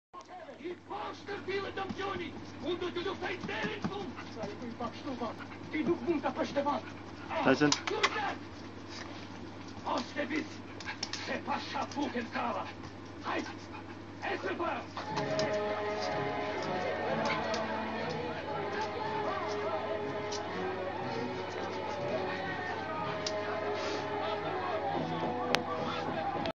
TYSON Teddy Humping. Funny.3GP
This is TYSON - dry Humping his Favourite Teddy Bear.